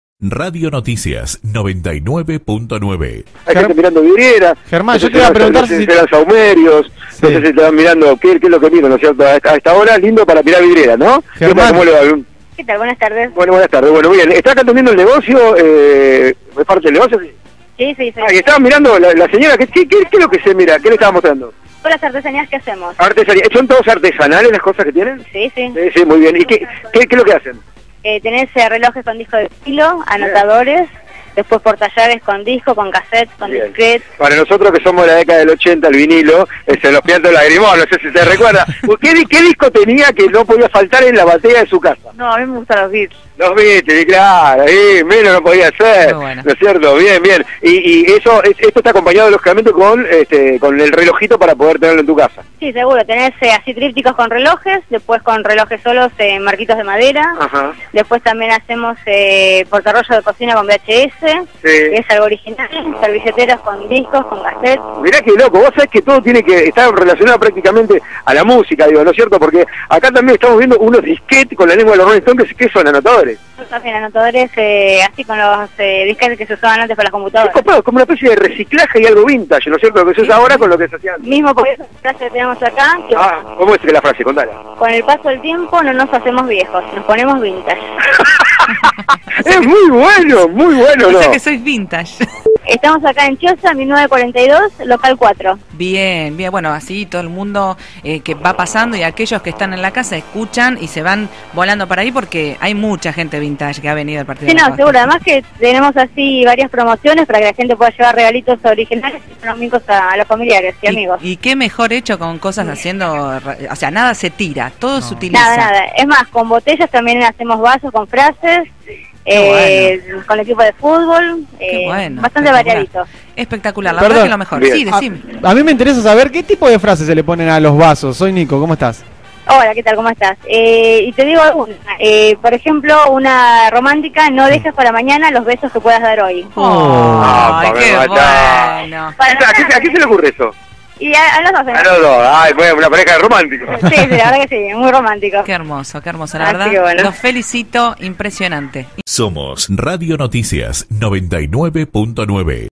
El movil llegó a las artesanias vintage
Germán habló con los propietarios de un local de artesanias vintage que se ofrecen a los visitantes objetos elaborados con objetos de los años 80.